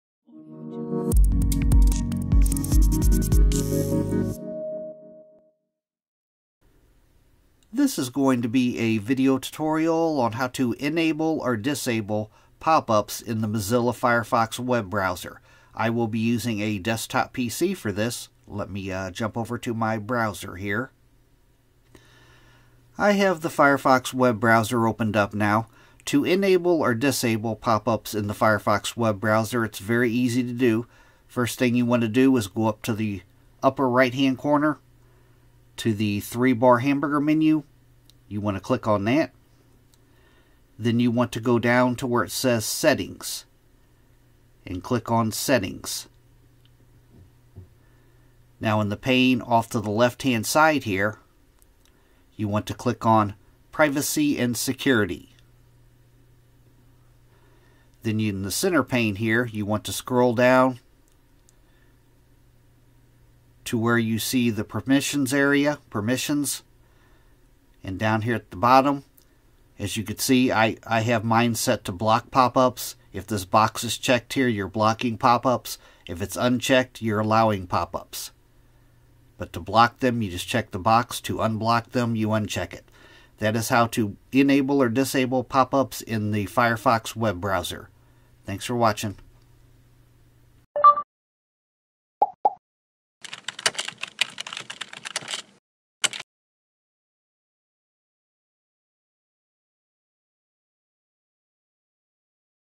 How To Enable Or Disable Pop-Ups In The Firefox Web Browser | PC Tutorial
Mozilla Firefox: How To Enable Or Disable Popups In The Firefox Web Browser | PC Tutorial

This is a video tutorial on how to allow or block pop ups  in the Mozilla Firefox web browser. I will be using a desktop PC for this tutorial.

Simple Steps
1. Open Mozilla Firefox.
2. Click on the 3 line hamburger menu in the upper right corner and choose "Settings".
3. Select "Privacy & Security" from the left side pane.
4. Scroll down to where you see the "Permissions" section and check or uncheck "Block Pop-Up Windows".

Chapters
0:00 How To Enable or Disable Popup Windows In Firefox
0:19 Open Mozilla Firefox
0:30 Select The 3 Bar Hamburger Menu
0:40 Select Settings
0:50 Select Privacy And Security
1:00 Scroll Down And Uncheck "Block Pop-Up Windows"
1:10 How To Block Popups In Firefox